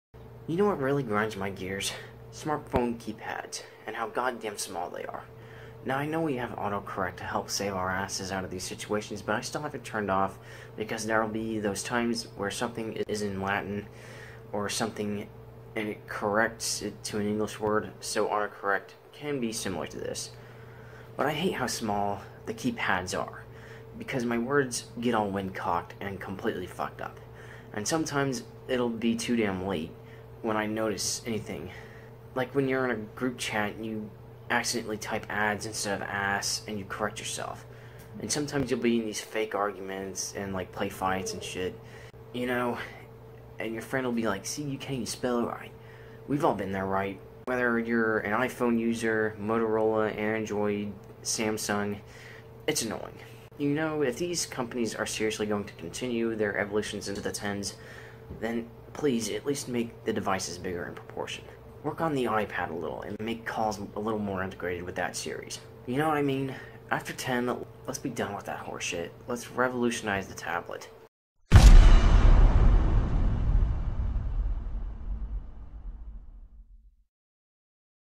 Grind My Gears: Smart phone keypads
A short rant. Instead of just wasting resources every year on new phones, let's just integrate it with tablets. 5 in x 3 in just isn't cutting it. And it's not just the keypad.